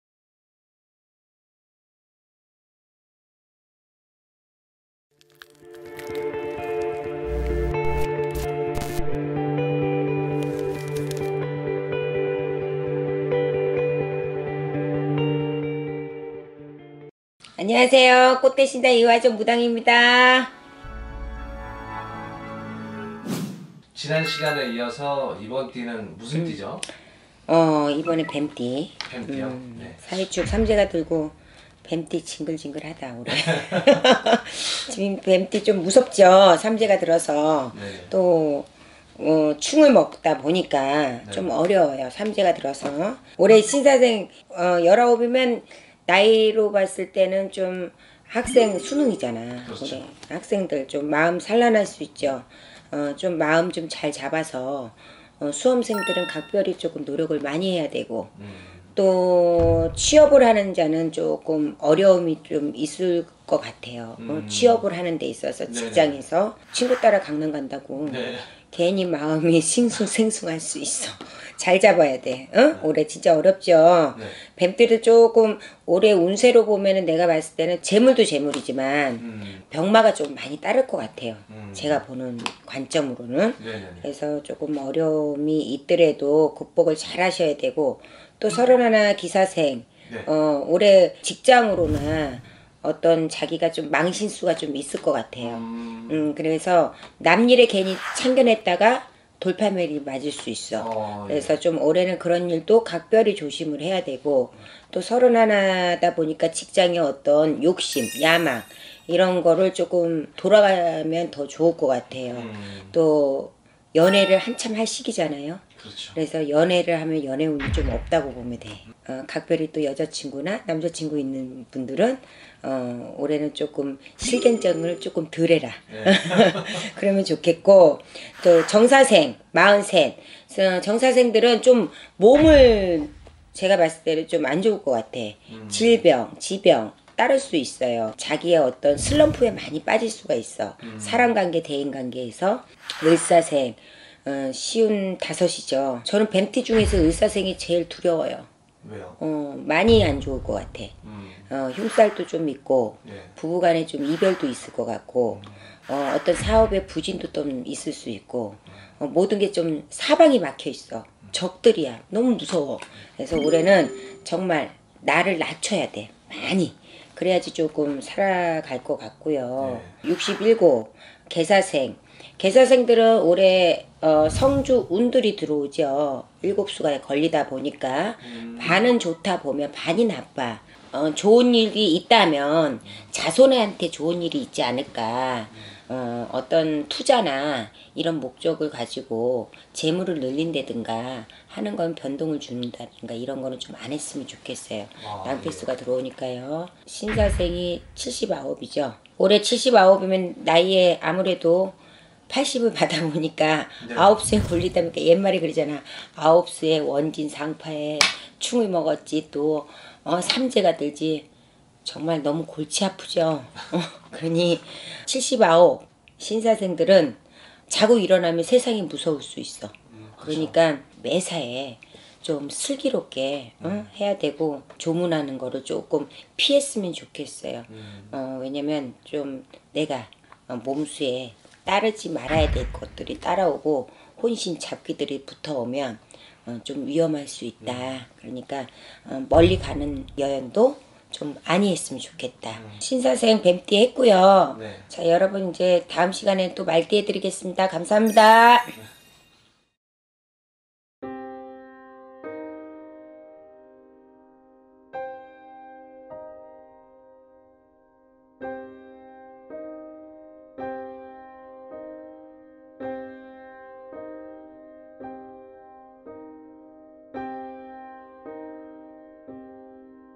2019년 기해년 뱀띠 나이별 신년운세 토정비결! 뱀띠는 놓치면 후회할 이야기! [용한점집 꽃대신당]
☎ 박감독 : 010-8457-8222